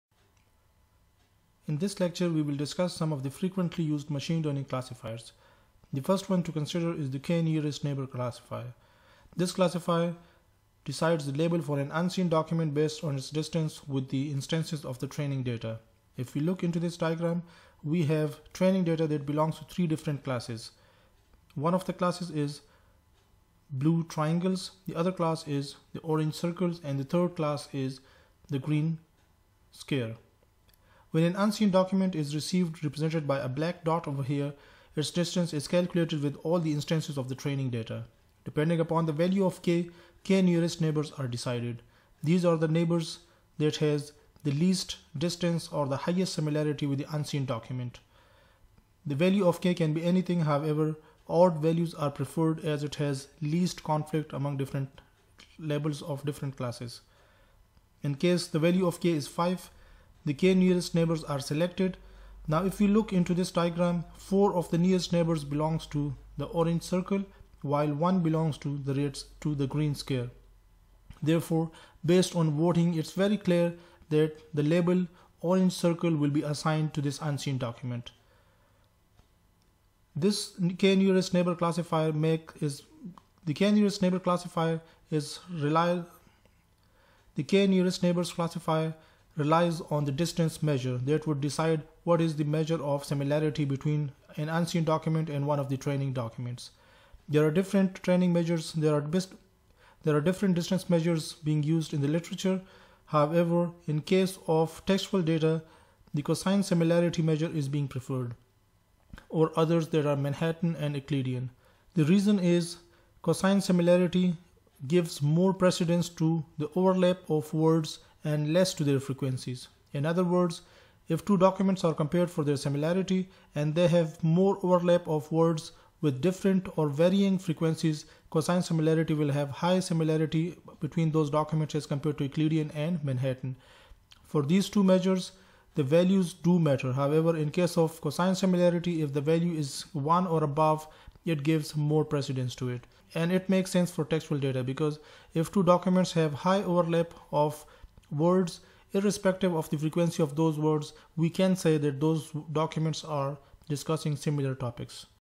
K-nearest neighbors classifier, KNN
In this video, we discuss the working of the K-nearest neighbors classifier. It's an instance-based classifier that doesn't train a model and, therefore, consults the training data each time it has to classify an unseen instance. It is also a distance-based classifier and therefore, a distance measure has to be decided for use with the KNN algorithm.